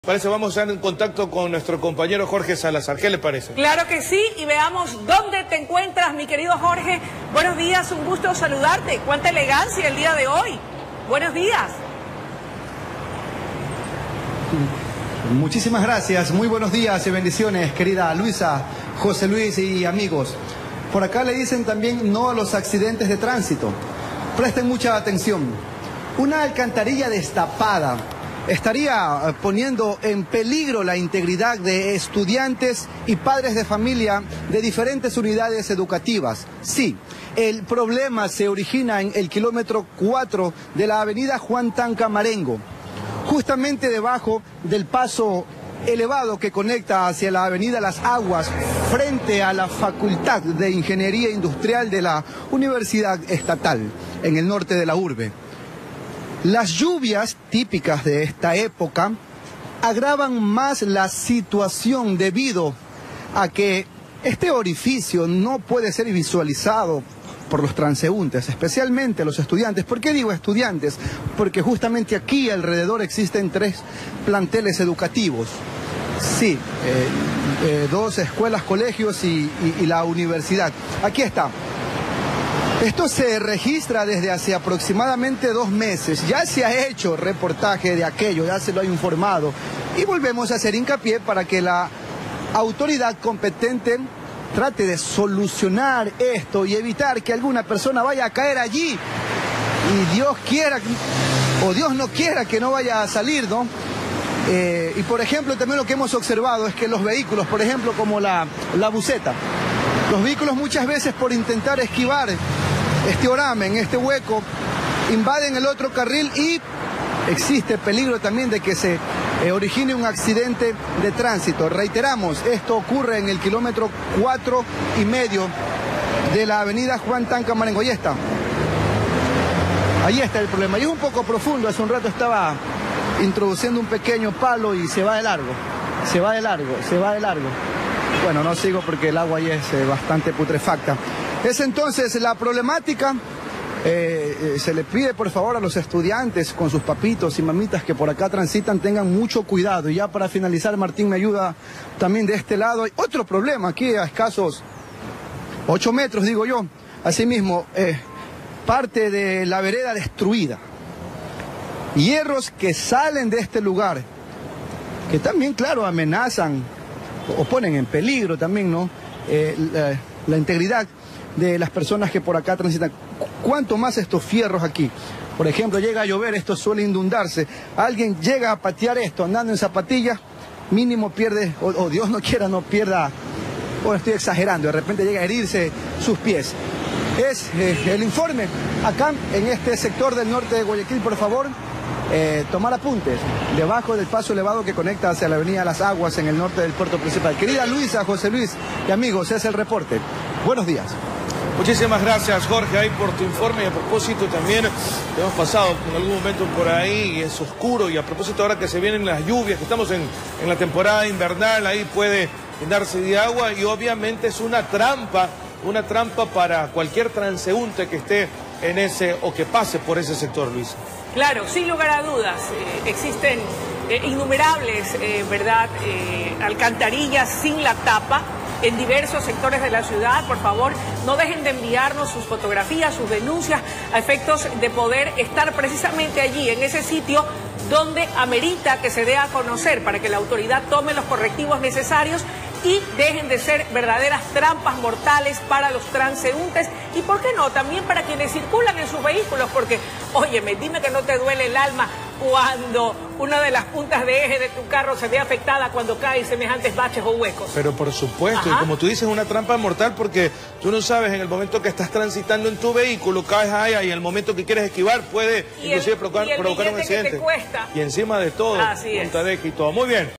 Una alcantarilla destapada pone en peligro a los conductores
La problemática se presenta en un sector de la avenida Juan Tanca Marengo

Los usuarios de ese vía le hacen un llamado a las autoridades para que tomen cartas en el asunto.